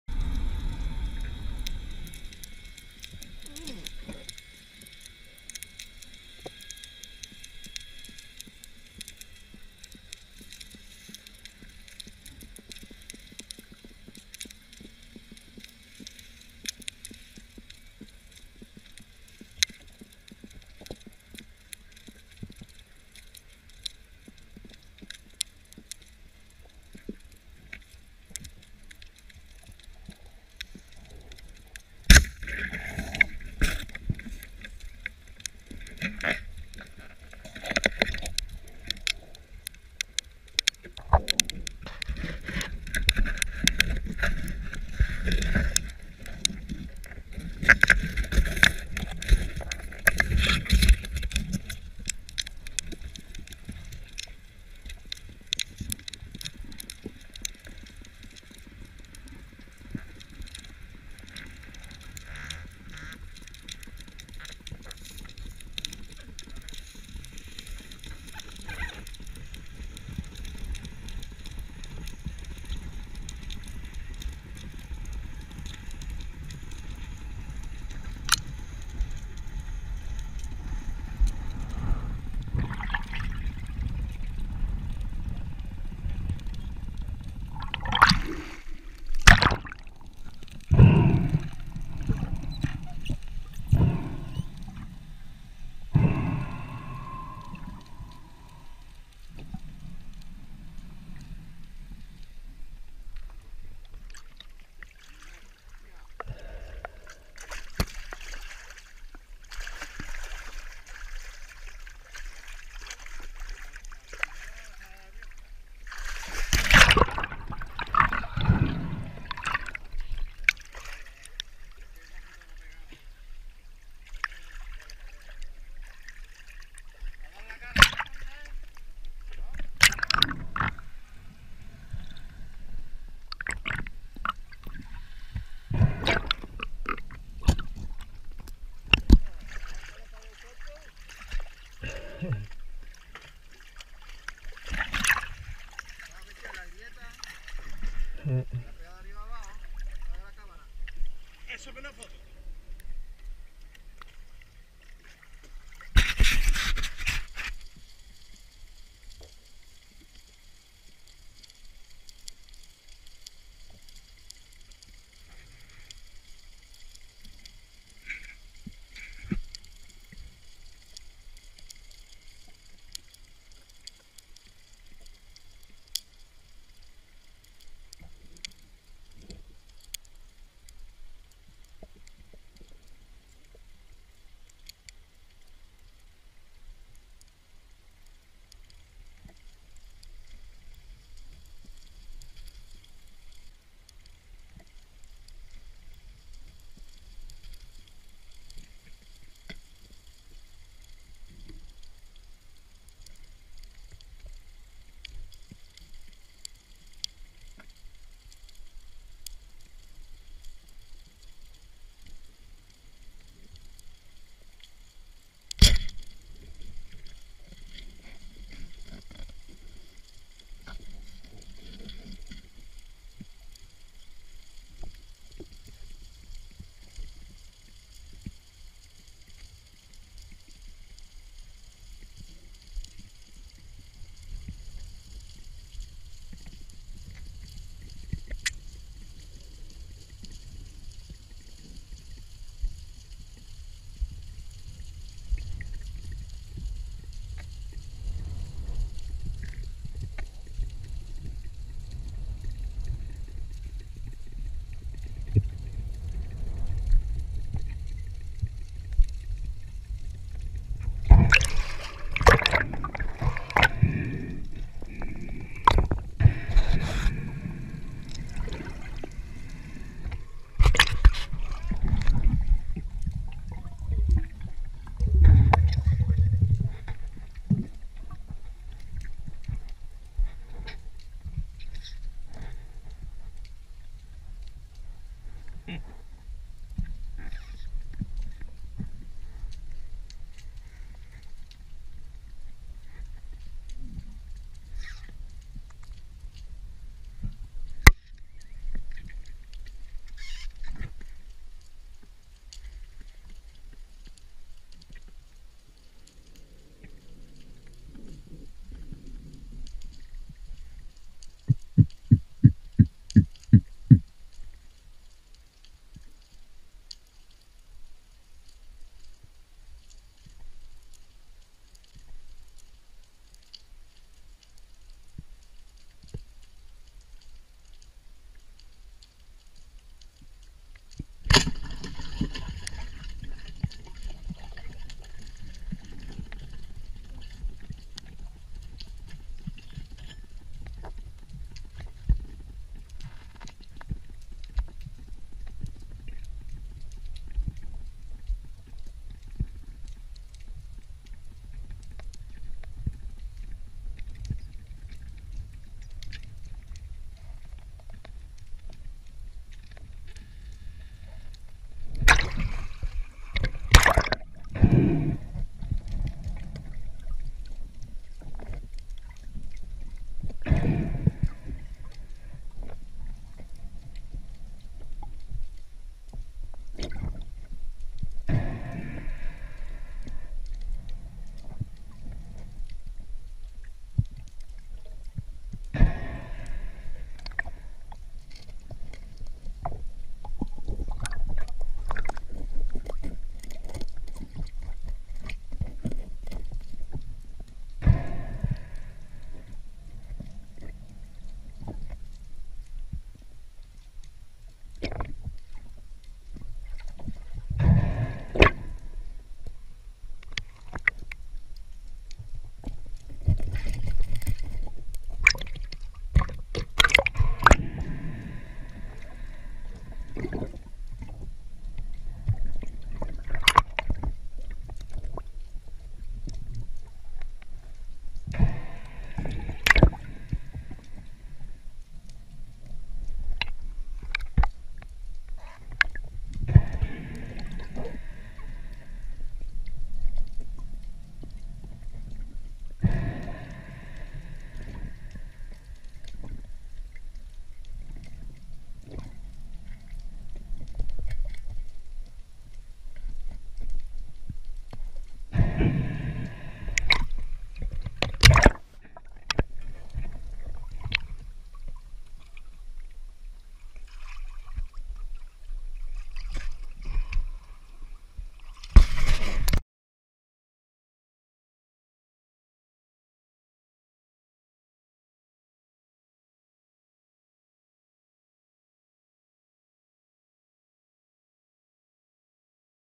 mero y abadejo a 34 metros y sargo real por José Miguel Lopez Corrales del team Pathos
vídeo en el que vemos a José Miguel Lopez Corrales realizando pesca submarina y nos enseña unos buenos lances y el final de un sargo real a la barbacoa.